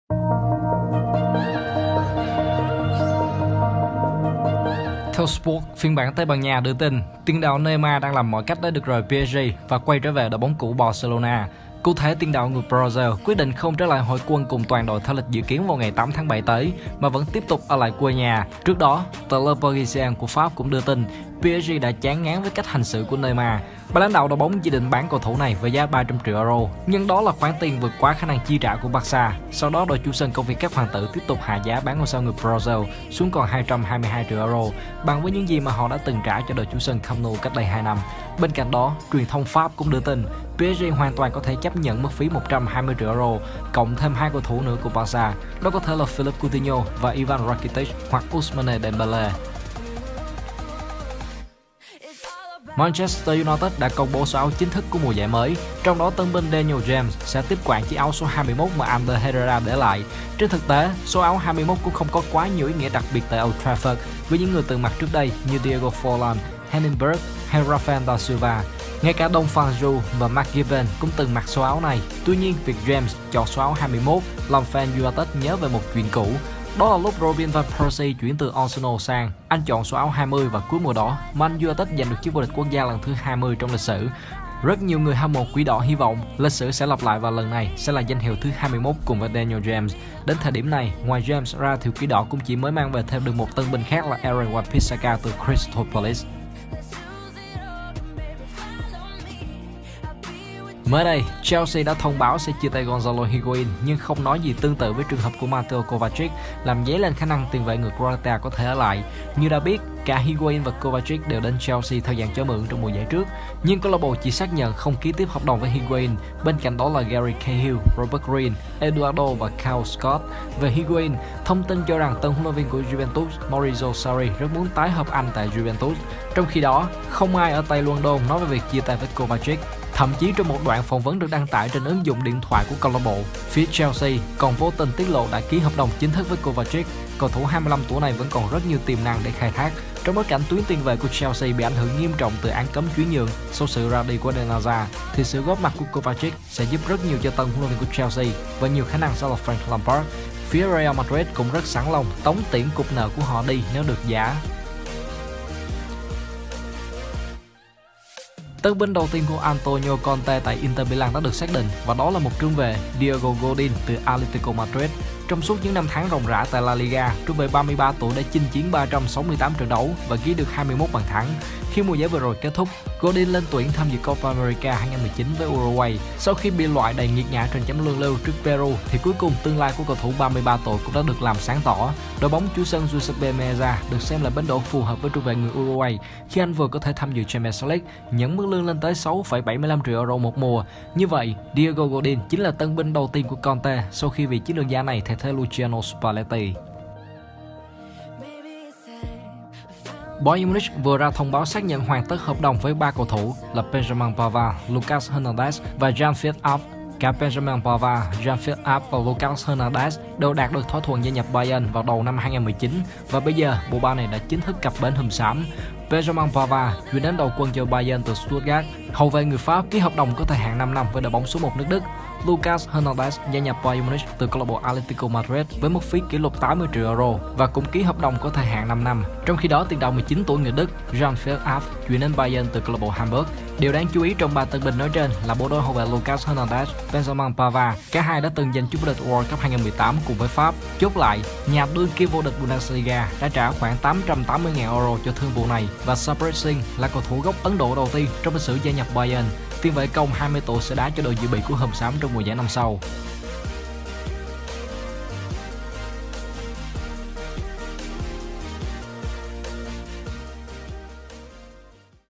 Điểm tin 90+ ngày 1/7 | Neymar nổi loạn, Inter công bố tân binh đầu tiên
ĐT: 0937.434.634